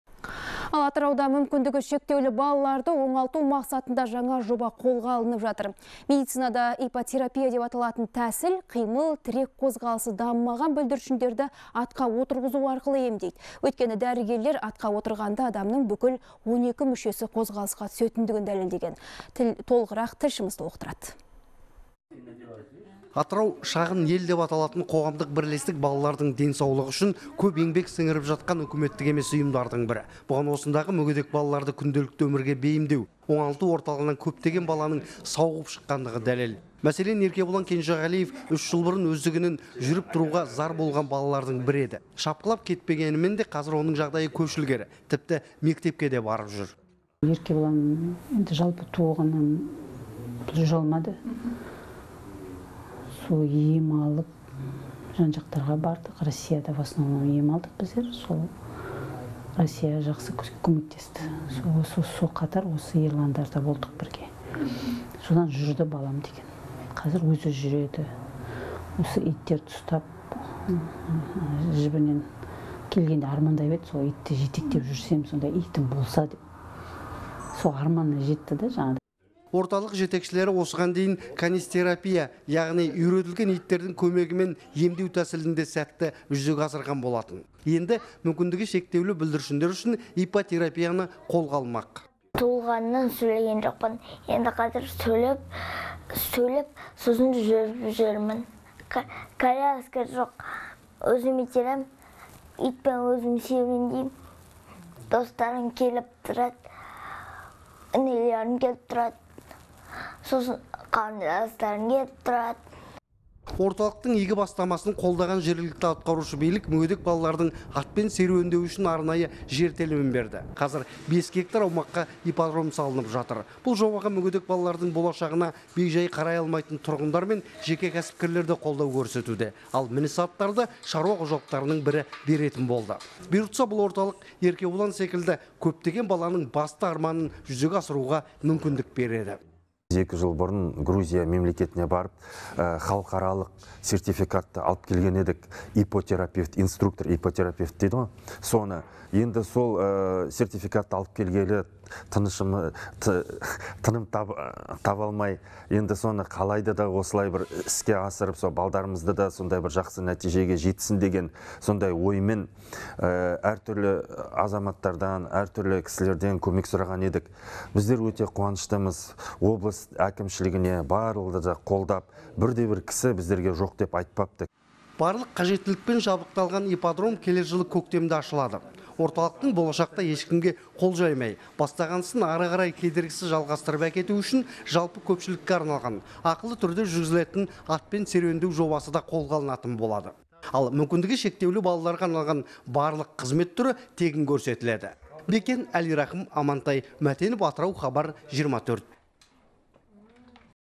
Атырауда ерекше балаларға арналған ипподром ашылмақ
Атырауда мүмкіндігі шектеулі балаларды оңалту мақсатында жаңа жоба қолға алынып жатыр.

Медицинада иппотерапия деп аталатын тәсіл қимыл-тірек қозғалысы дамымаған бүлдіршіндерді атқа отырғызу арқылы емдейді. Өйткені дәрігерлер атқа отырғанда адамның бүкіл 12 мүшесі қозғалысқа түсетіндігін дәлелдеген.